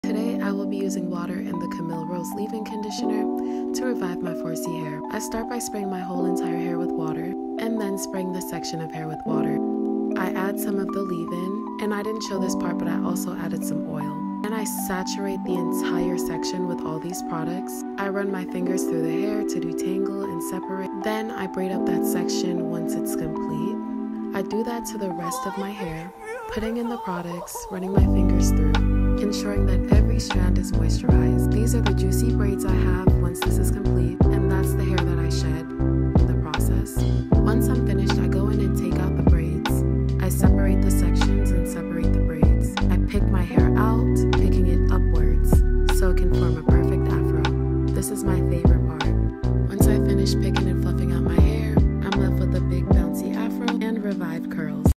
How to moisturize extremely Dry 4C hair & Style A Perfecf Afro
HEY Y’all, I always get asked about maintaining my hair during the week. I wear my hear in an Afro ALL the time, this means that I have to be careful and moisturize my hair every 4-5 days(so it doesn’t become too dry or tangled together).

In this video I used a water, a leave in conditioner, and Vaseline, to moisturize and detangle my 4C hair. I sectioned my hair and put it into braids once the sections were moisturized. When I take down the braids at the end my hair is softer, fluffy, and overall looks more healthy.